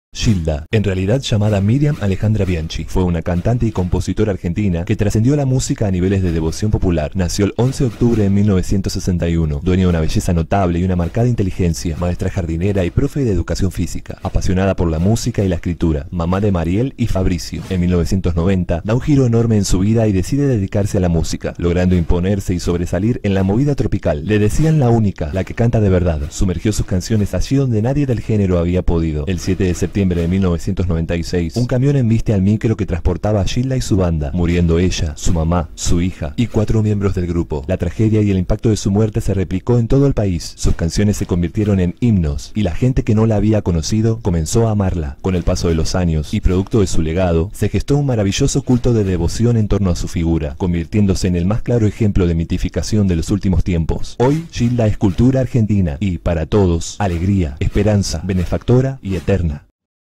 ¿QUIÉN ES GILDA? EL FENÓMENO Y LO QUE REPRESENTA
¿QUIÉN ES GILDA?
Te lo explicamos en un minuto.

A 25 años de su paso a la inmortalidad, hicimos este video para que puedas contarle al mundo brevemente quién fué y es Gilda, el fenómeno y todo lo que significa para la gente que la ama y la recuerda con pasión.

¿Qué más es Gilda para vos? ¡CONTANOS!

¡COMPARTILO!

Adorable Gilda

ADORABLE GILDA, DONDE ESTÁ LA MAGIA ❤